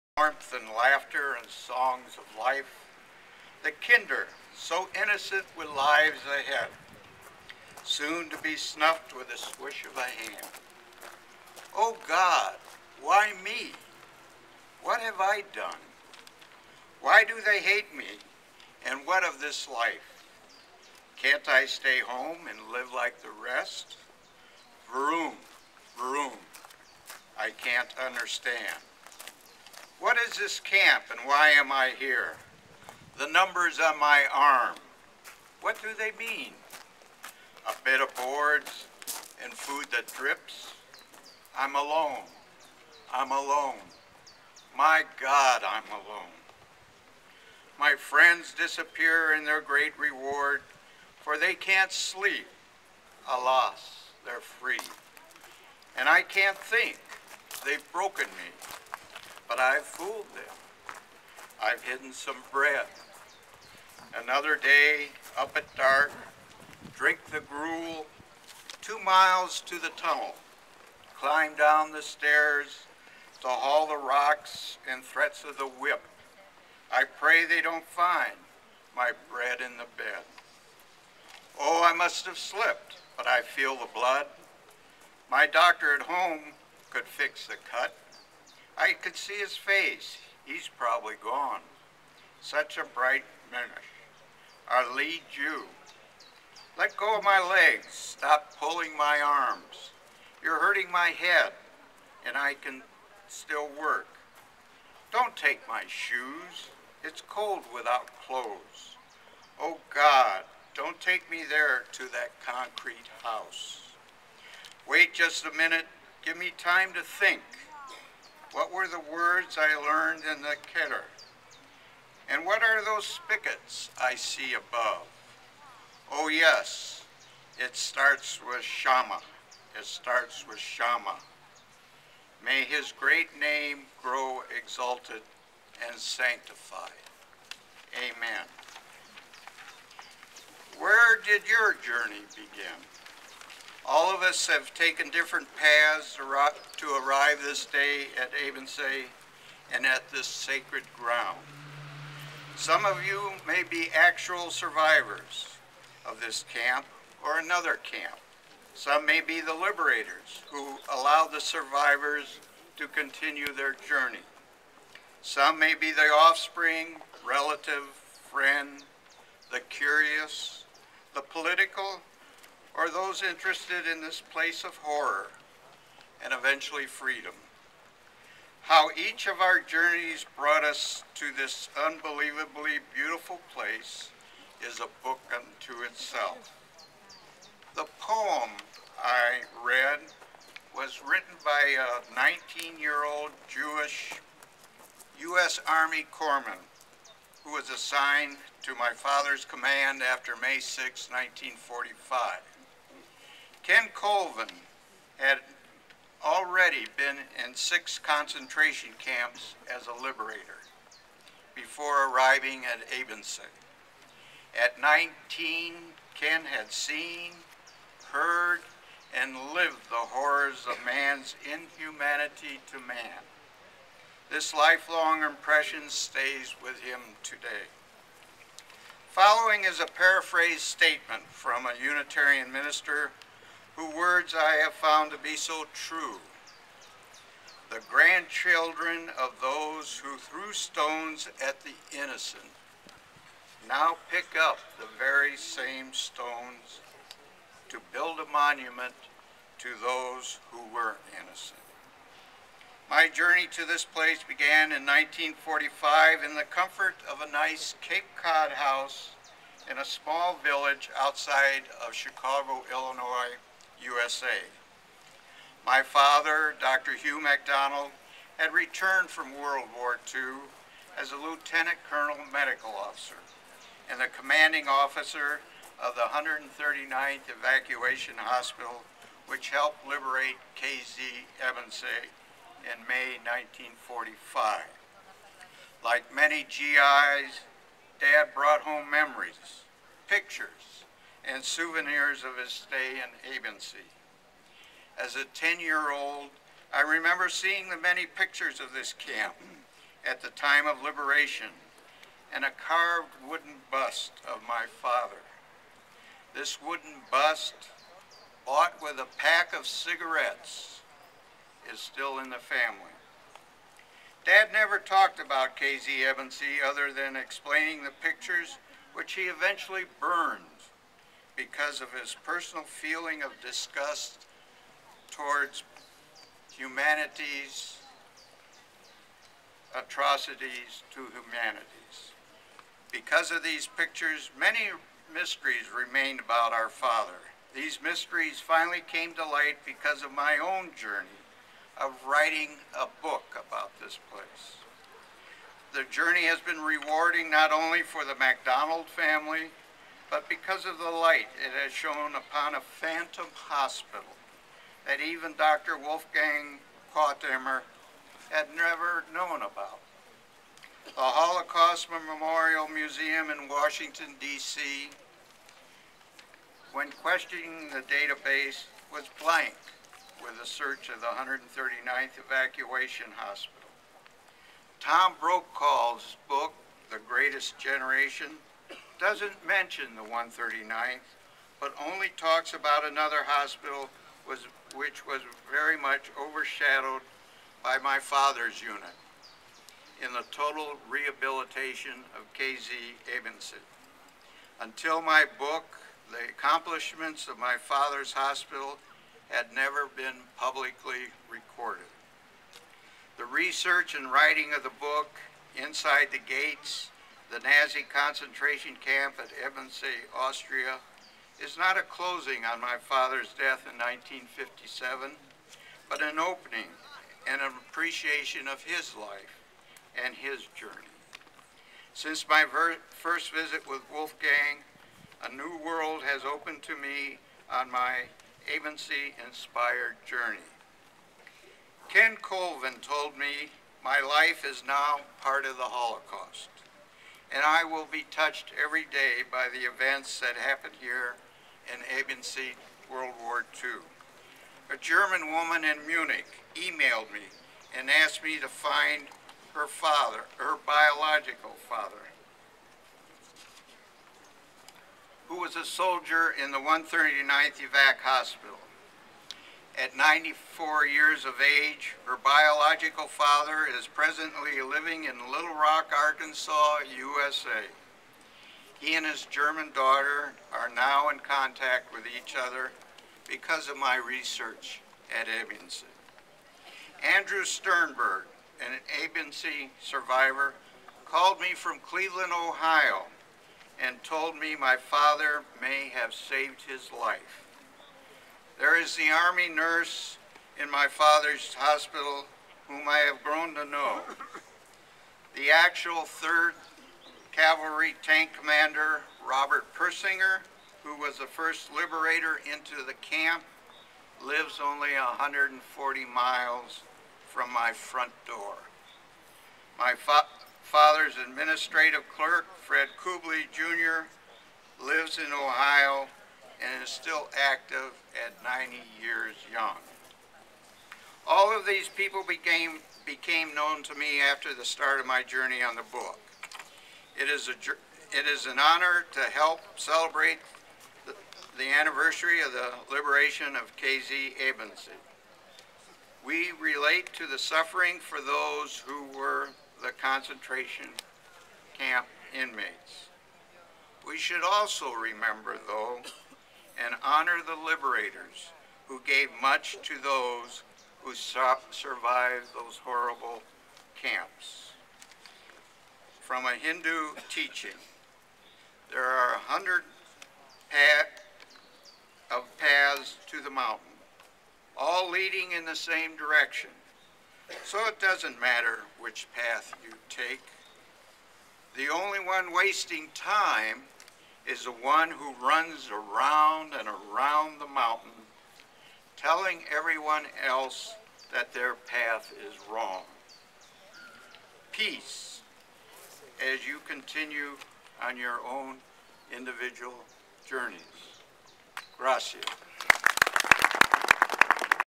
Liberator's son talking at liberation ceremony of concentration camp
Speech at ceremony on May 7, 2011, by only American, Dr. Richard G Macdonald, for the celebration of the liberation of the concentration camp at Ebensee Austria which happened on May 6, 1945.